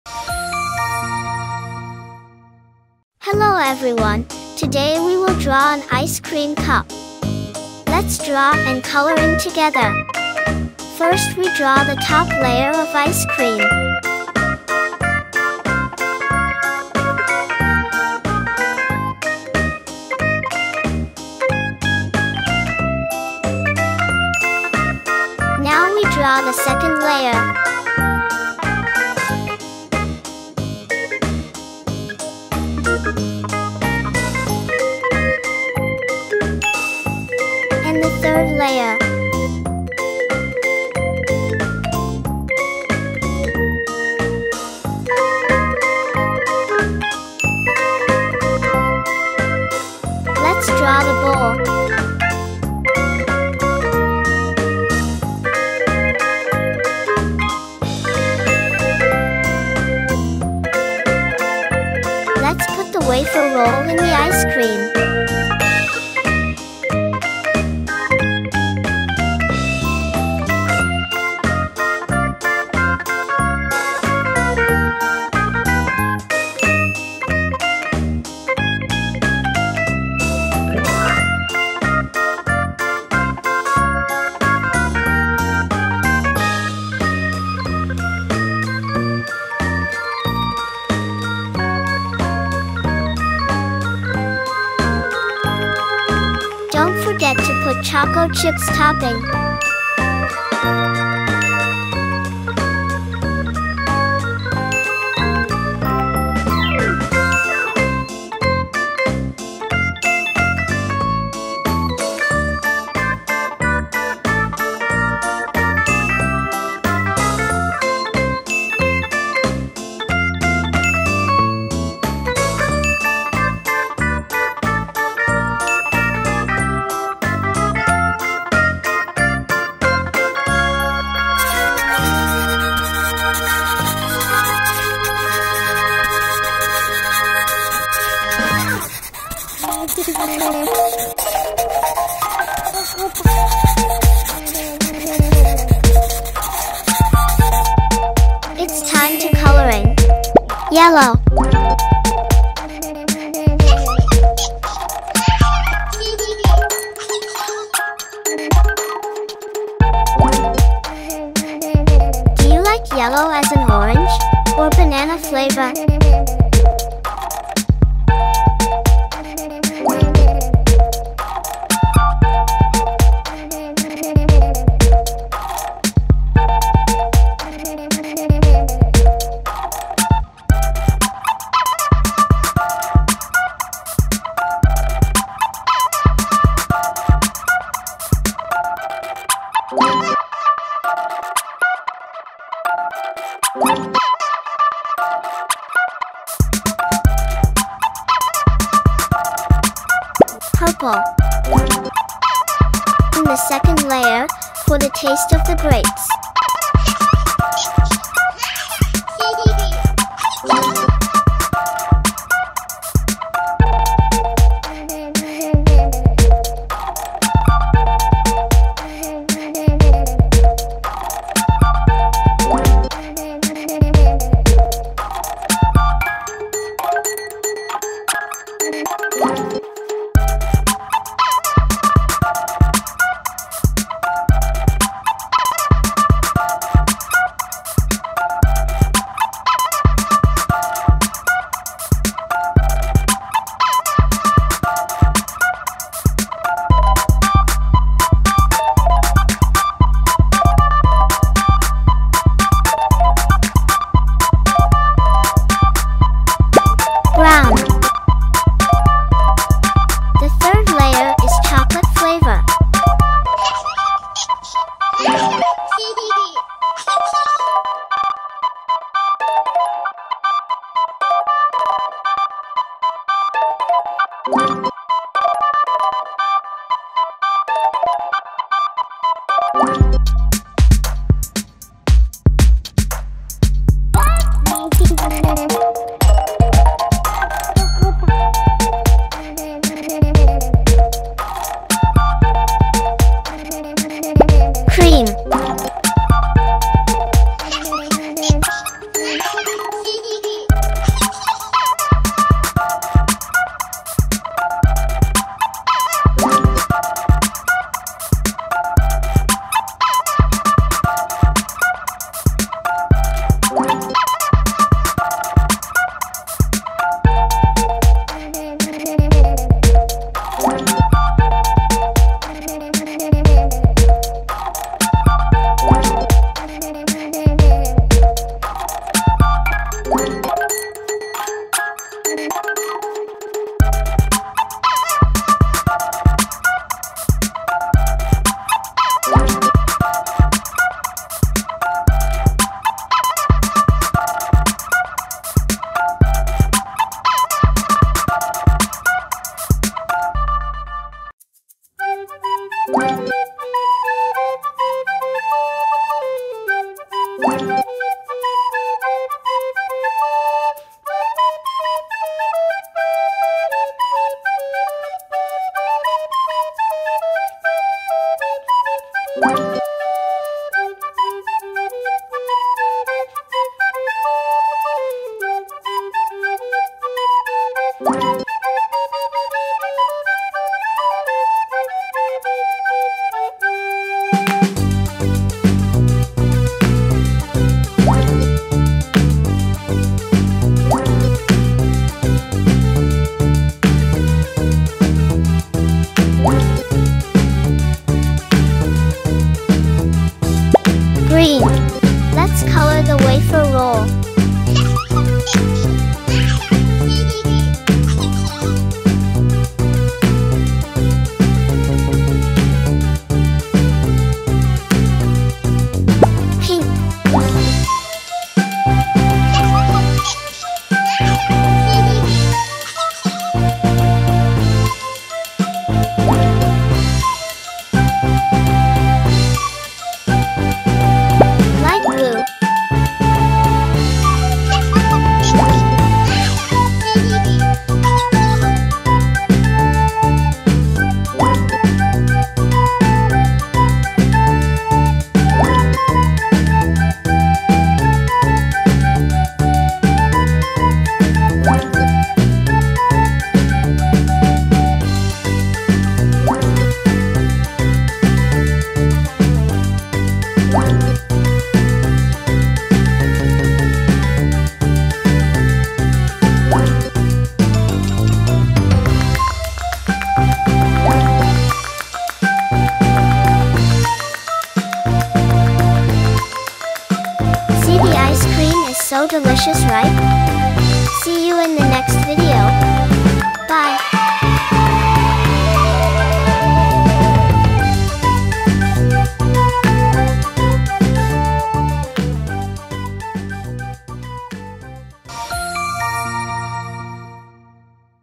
Easy Drawing an Ice cream for kids, step by step Painting & Coloring for Toddlers | How to Draw
Learn How to Draw an Ice cream
Easy Drawing Ice cream step by step.
Easy, simple drawing Ice cream for Kids.
How to draw Ice cream step by step easy drawing tutorial.